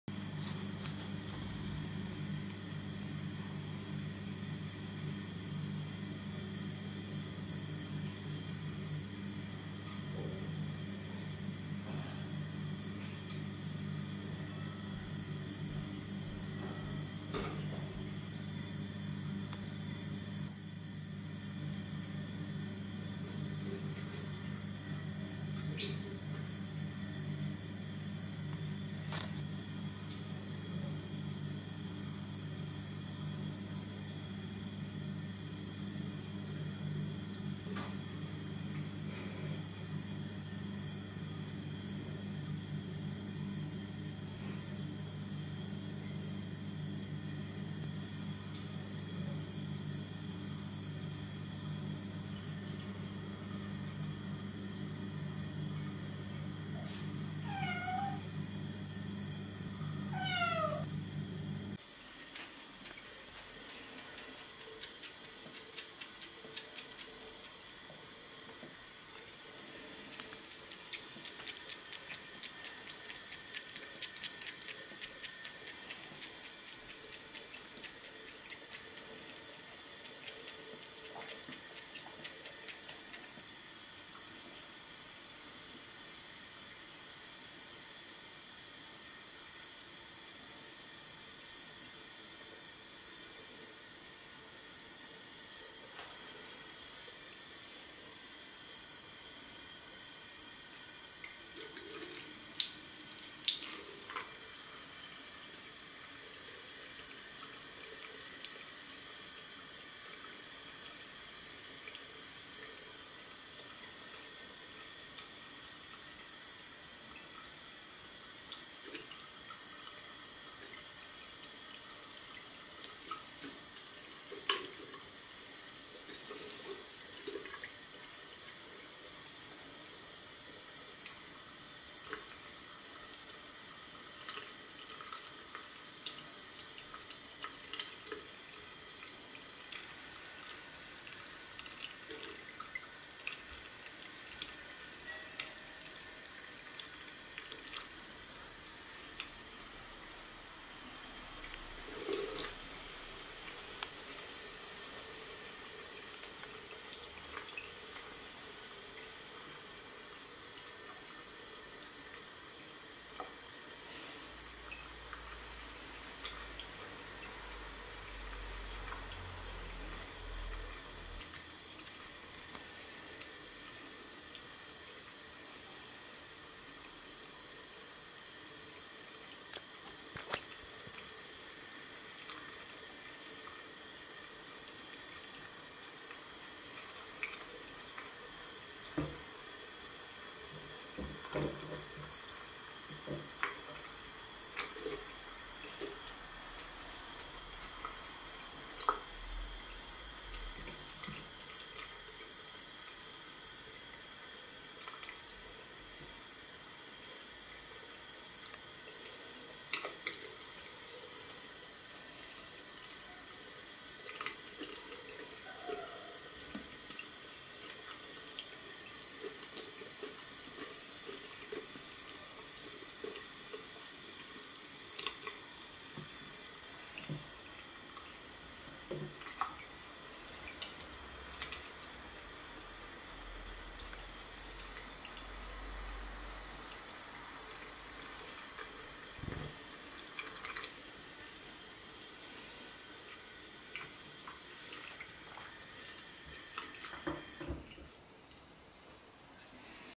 cute kitten video bathroom play she discovers water Chloe the Exceptional Cat 1 November 2014
29 days after we adopted semi-feral cute kitten Chloe, the Exceptional Cat  is now about 3 months old.

Her confidence and gymnastic skills have improved and now she can jump onto the bathroom hand basin.

She quickly discovers the magic of running water, slippery ceramic and how hard it is to eat a mixer tap.